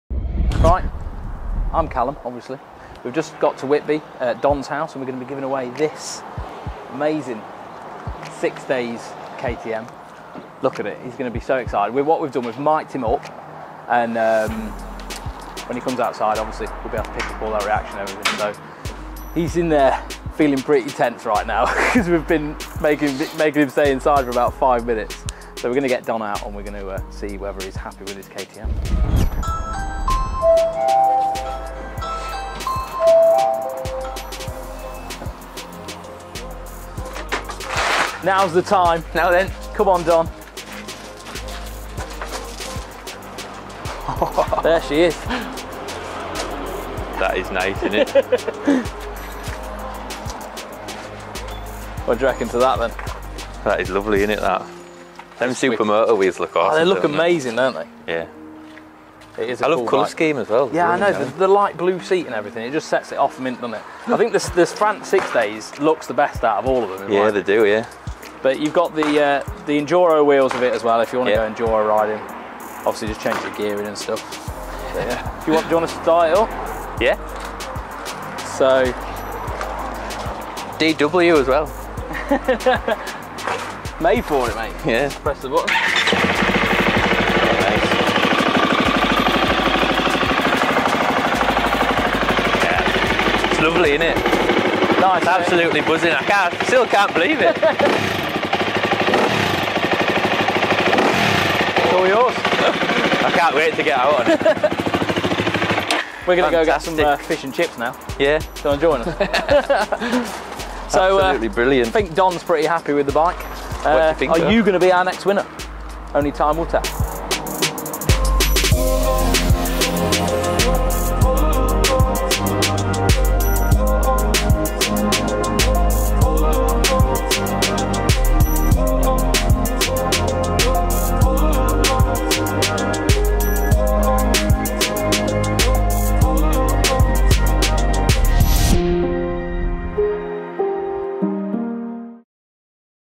Giveaway Life Winner! Don Walker - KTM 6 Days France Edition
Win your Dream Bike with Giveaway Life | Enter our Bike, Car and Tech Competitions from just 99p | You could be our next winner!

Giveaway Life winner Don Walker who scooped the KTM 6 Days France Edition with ticket number 33 had is new bike delivered to his home in Whitby! Don won the KTM on Sunday 7th Feb and had it delivered at his earliest convenience – congratulations once again Don!